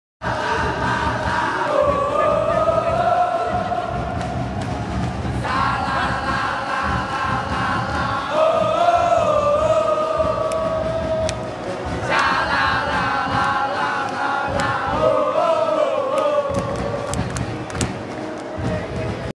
DERBY PSIS vs PERSIS
DERBY PSIS vs PERSIS @Nanorame

Pertandingan level tinggi akhirnya dimenangkan oleh PSIS Semarang dengan skor 2-1 hasil gol dr sundulan Dewangga stlh menerima assis dr Fredyan ucil dan sundulan Carlos Fortes jg assist dr Fredyan Ucil sedangkan gol Persis Solo hasil sundulan Fabiano Beltrame hasil assist lemparan ke dalam Aaron Evans dr sisi kiri pertahanan PSIS yg mana penjaga gawang PSIS adalah Redondo.

Keperluan kerja sama /endorse atau komunikasi bisa menghubungi hp /wa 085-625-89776.